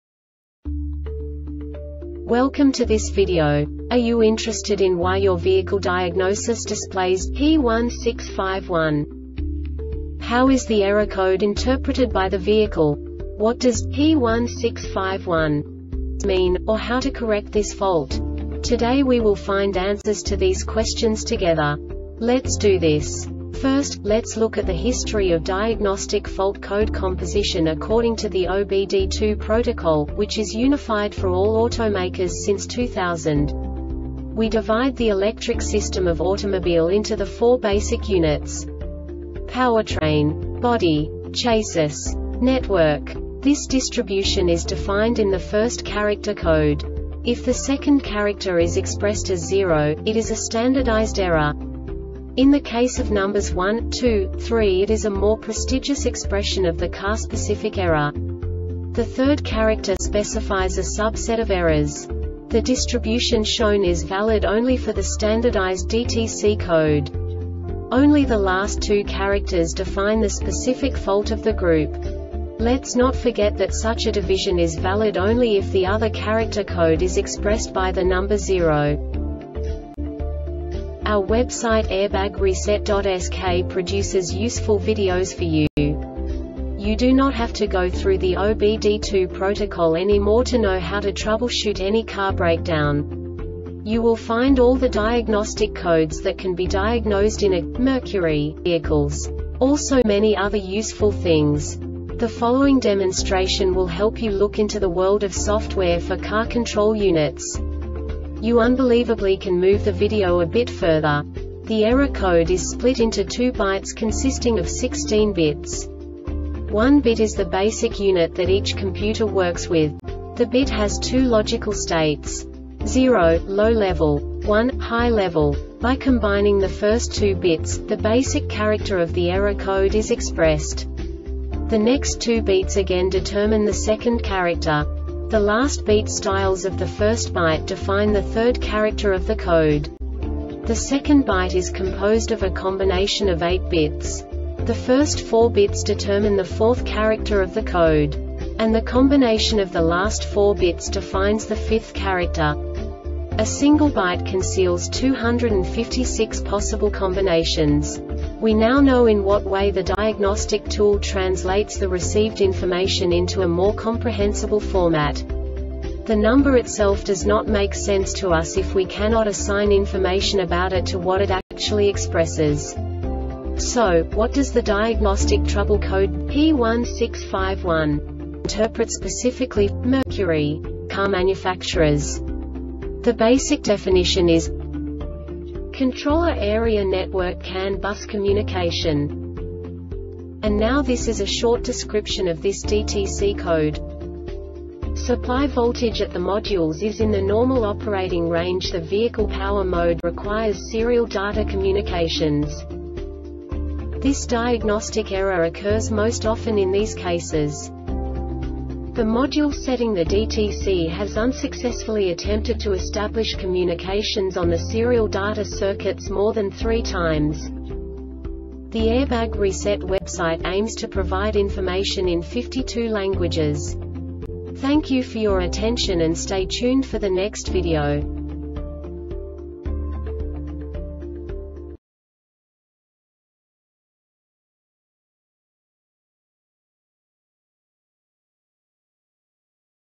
DTC Mercury P1651 Short Explanation
Contents:
0:21 Basic DTC analysis according to OBD2 protocol standard.
1:48 Insight into programming
2:58 A diagnostically understandable description of the Diagnostic Trouble Code
3:05 Basic error definition

Make
Mercury
Code
P1651
Definition
Controller Area Network CAN Bus Communication
Description